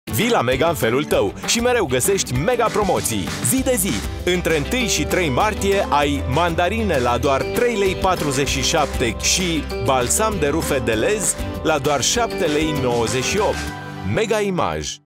Mega Promo zi de zi - promoții 1-3 martie | MEGA IMAGE
O întreagă serie de promoții pentru o lună întreagă de răsfăț binemeritat, doar la Mega-ul tău.